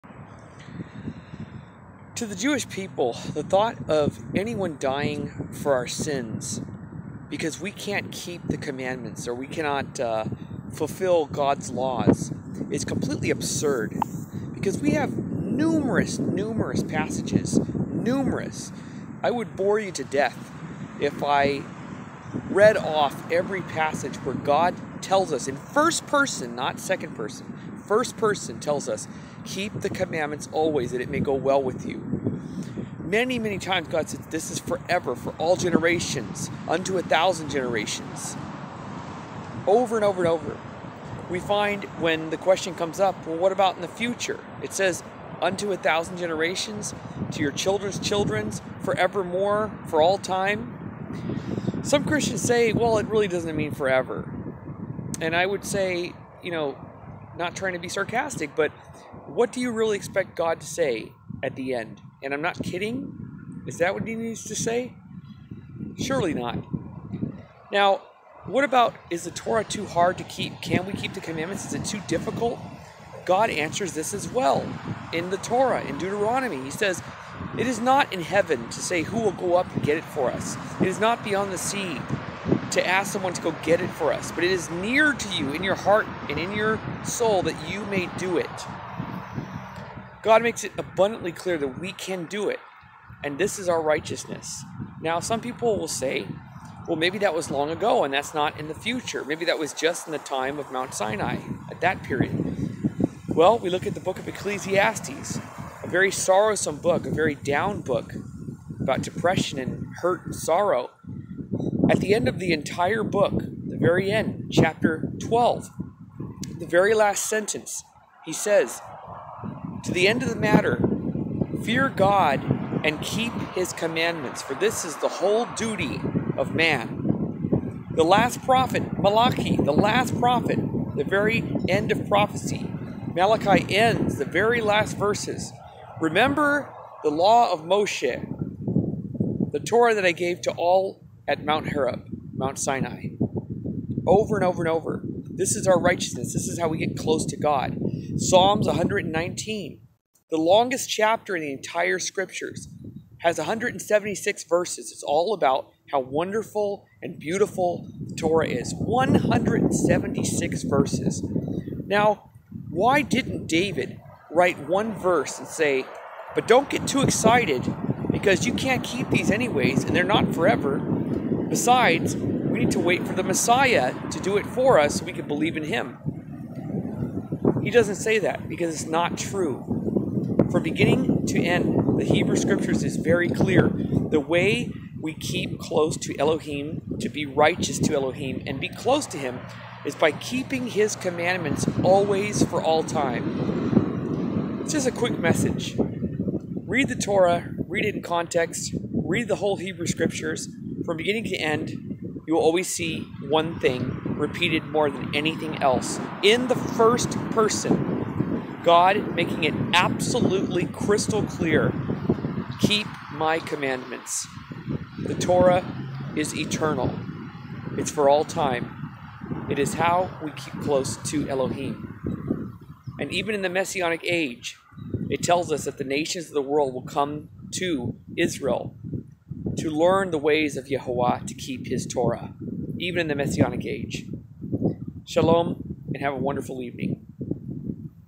Torah is Eternal (Commandments)
Karaite Judaism Teacher of Tanakh Peleh Ben Avraham talks about the Clear...very clear message in TANAKH - on how to keep close to our Creator...The Torah is for our benefit and our guide to live a righteous life and to live according to the way GOD wants us to live.